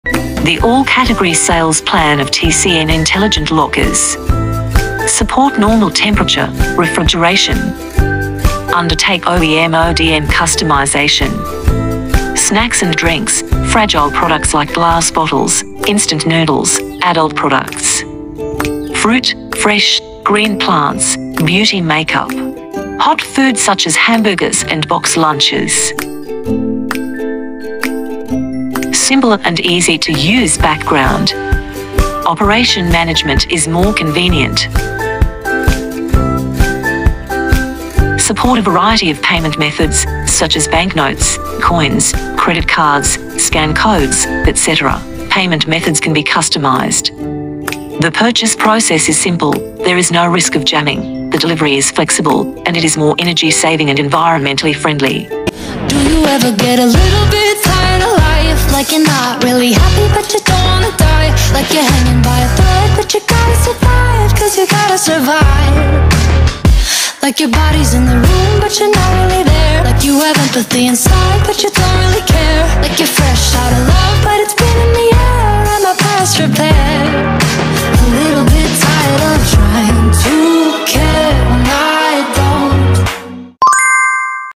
TCN Locker Vending Machines
TCN combines a locker system with a vending machine for the ultimate automated retail solution! A great way to expand your business, instead of opening a staffed brick and mortar store.
A locker vending system will stay open 24/7. You can place them in new markets and sell all kinds of goods you want. With no additional staffing costs, it is a fast, fun and safe way to reach more potential customers.
Highlights:
·Undertake OEM/ODM customization
·Different payment methods available
·More energy-saving and environmentally friendly